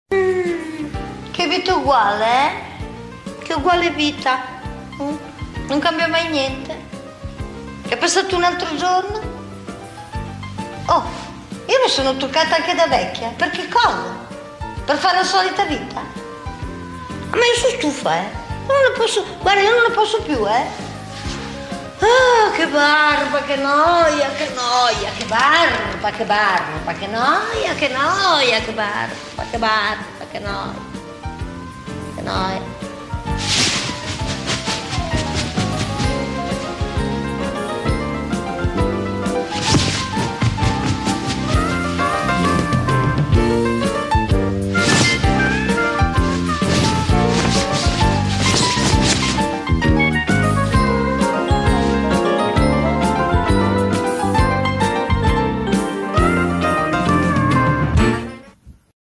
Casa Colombo - omaggio ai mitici Sandra e Raimondo in Casa Vianello
Piccolo omaggio ad uno dei più famosi sketch dei sempre unici Sandra Mondaini e Raimondo Vianello. Serena grandissima... ❤️